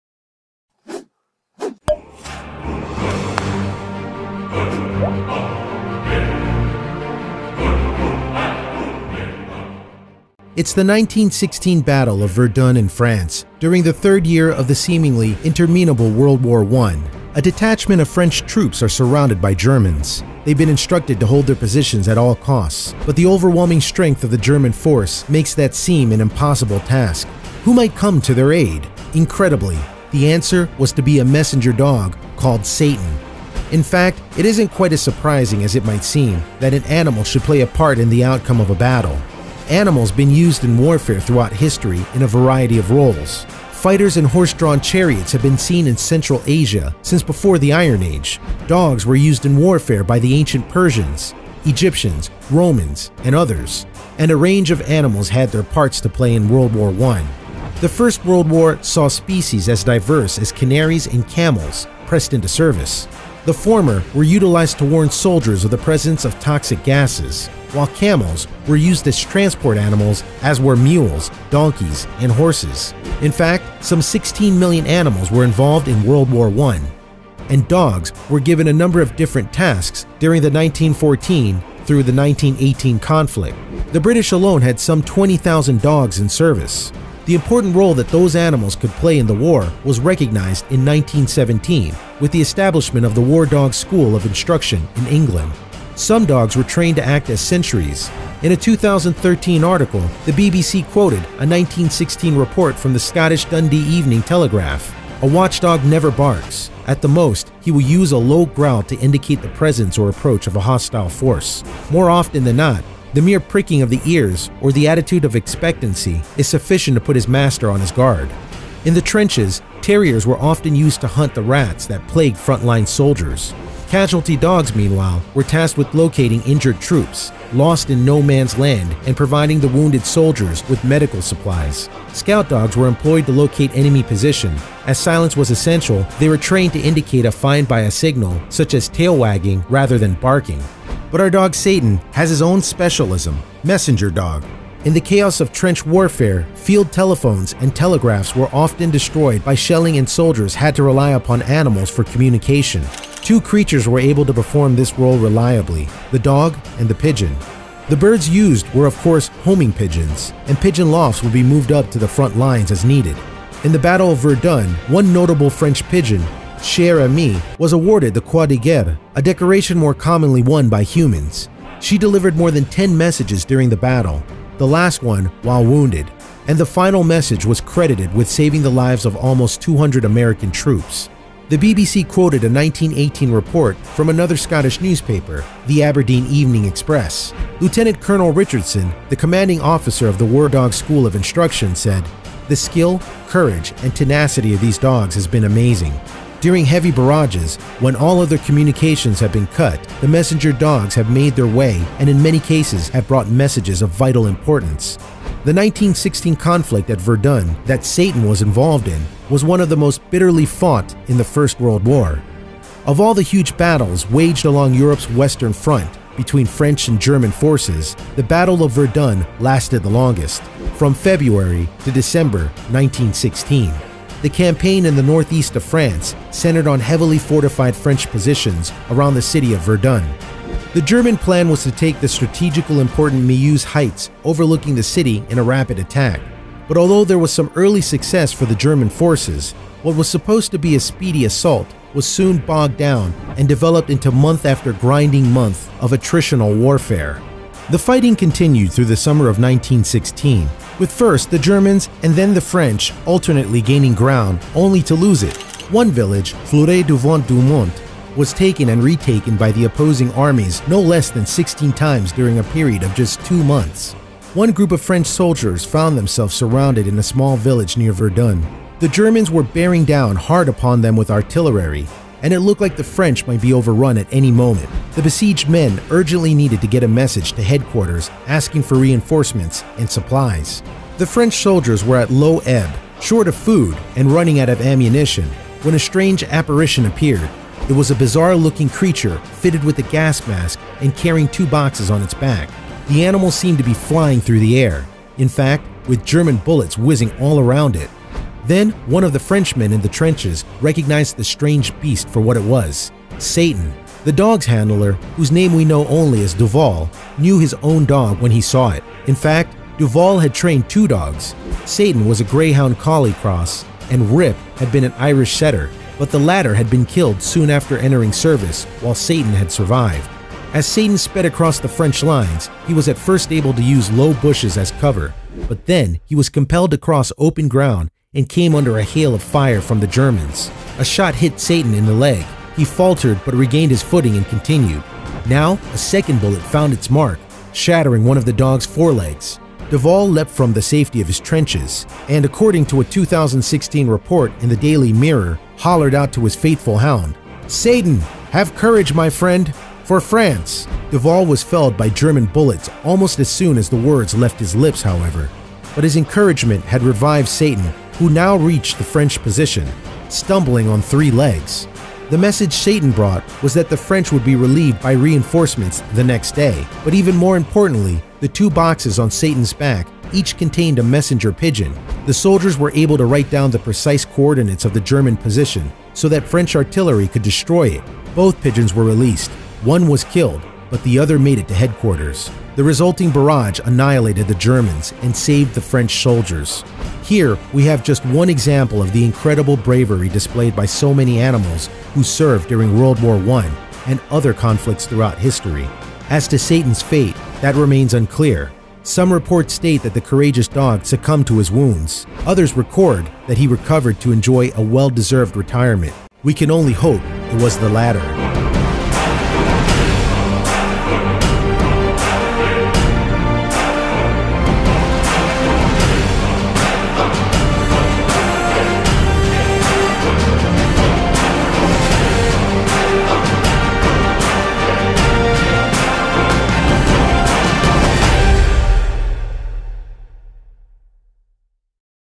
This Fearless Dog's Bluntness Astounded Everyone. Number Of Lives He Saved During WWI Is Phenomenal
It’s the 1916 Battle of Verdun in France during the third year of the seemingly interminable World War One. A detachment of French troops are surrounded by Germans. They’ve been instructed to hold their position at all costs. But the overwhelming strength of the German force makes that seem an impossible task. Who might come to their aid? Incredibly, the answer was to be a messenger dog called Satan.

In fact, it isn’t quite as surprising as it might seem that an animal should play a part in the outcome of a battle. Animals been used in warfare throughout history in a variety of roles. Fighters in horse-drawn chariots have been seen in Central Asia since before the Iron Age. Dogs were used in warfare by the ancient Persians, Egyptians, Romans and others. And a range of animals had their parts to play in World War One.

The First World War saw species as diverse as canaries and camels pressed into service. The former were utilized to warn soldiers of the presence of toxic gas, while camels were used as transport animals, as were mules, donkeys and horses. In fact, some 16 million animals were involved in World War One.

And dogs were given a number of different tasks during the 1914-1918 conflict. The British alone had some 20,000 dogs in service. The important role that those animals could play in the war was recognized in 1917 with the establishment of the War Dog School of Instruction in England.
We will remove the clip, video or come to an agreement. Thank you so much.